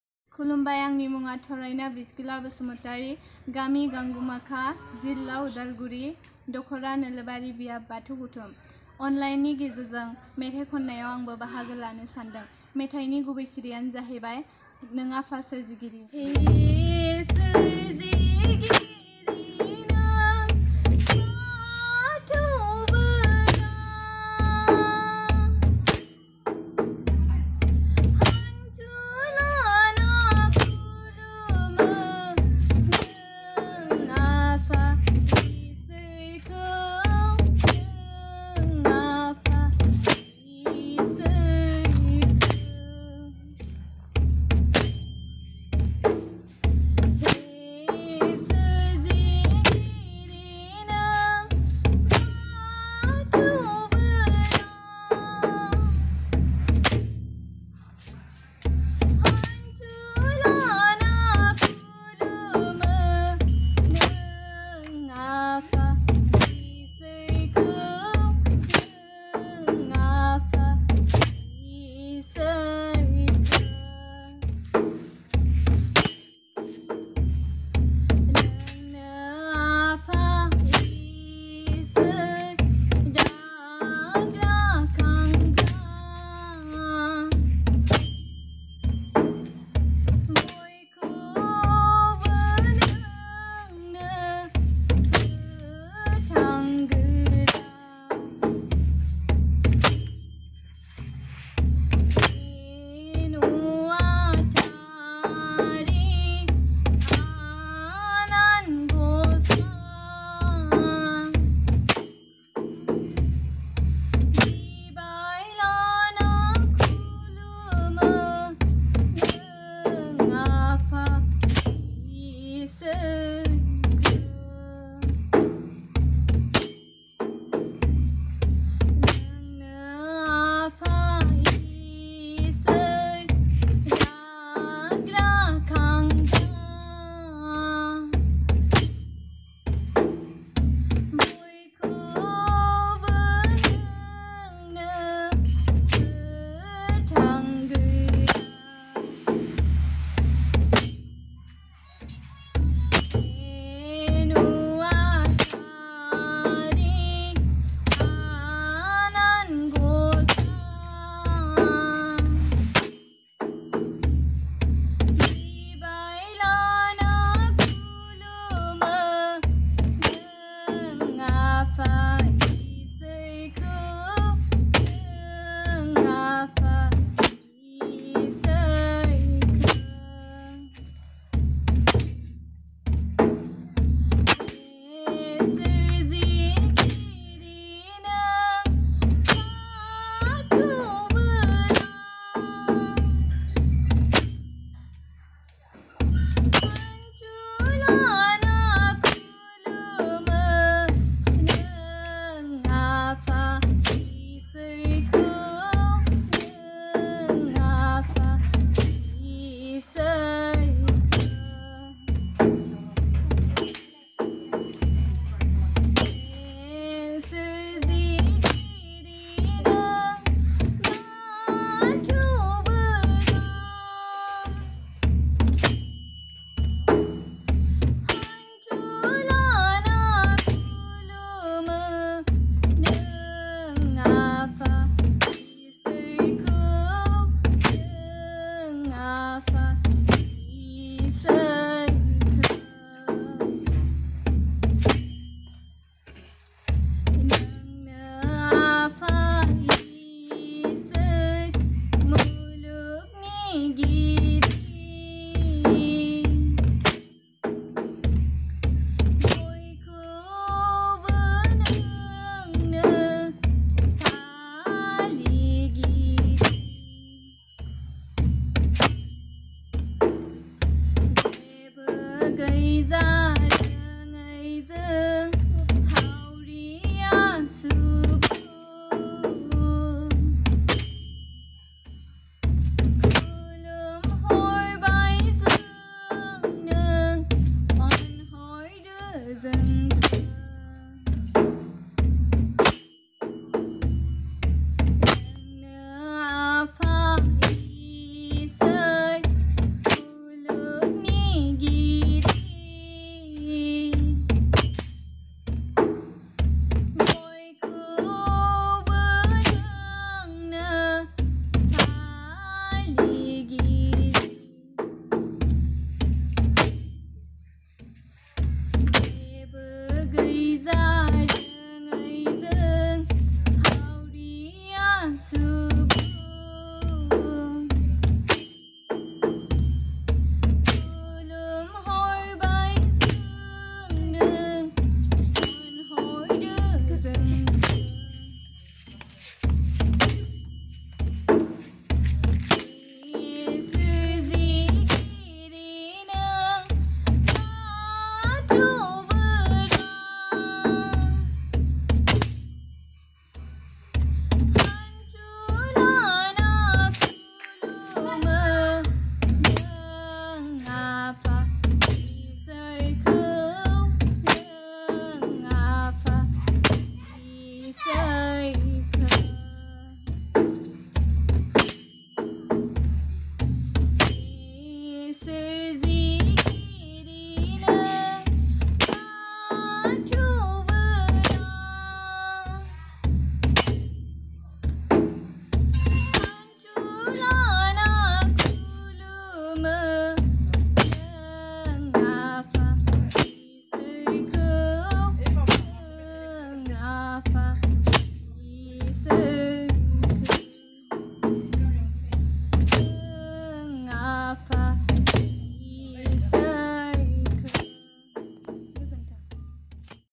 Briskila Basumatary/Contestant No-70/दिंखन-क/बाथौ आर'ज मेथाइ खननाय बादायलायनाय-2021/Age-17/Udalguri
बाथौ आर'ज  मेथाइ खननाय बादायलायनाय -    2021 :
  दिंखन - क     बादाय नं - 70 :
थरायना  ब्रिसकिला बसुमतारि  :
बैसो -  17 ,   जिल्ला - अदालगुरि ,  बियाब - नोलोबारि , गामि - गांगुमाखा :

    बान्जायग्रा - लाइमोन आरो माइनाव आफाद, दुलाराय बाथौ गौथुम :
  फोसावग्रा - बाथौ बिबुंसार Web - Portal :
     मुं थिसननाय खालार -
  01/08/2021 निफ्राय 20/09/2021
    बांसिनै मिथिनो सुलाद ला - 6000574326
       बान्था :-  सेथि - 15000 /- , नैथि - 11000 /- , थामथि - 7000 /- , थुलुंगा बान्था :-  सानै - 3000 /- साबायखर बान्था :- सा बा - 2000 /-
  👇👇👇 Link for PDF - Rules & Regulations -